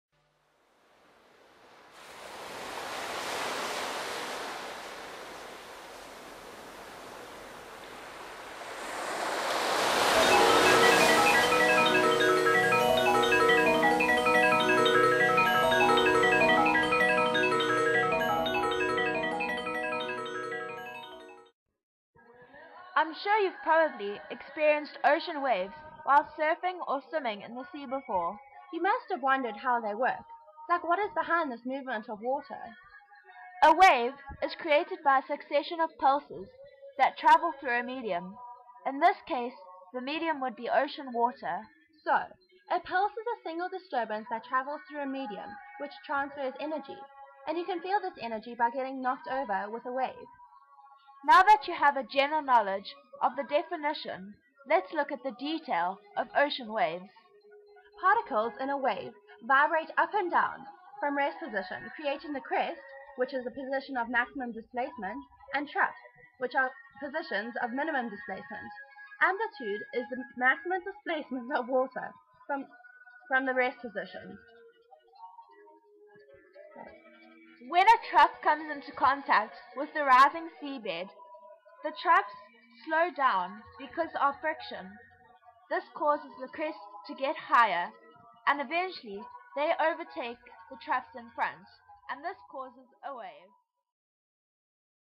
Waves - A Grade 10 Science video
A teaching video created by Grade 10 Science girls at Durban Girls' College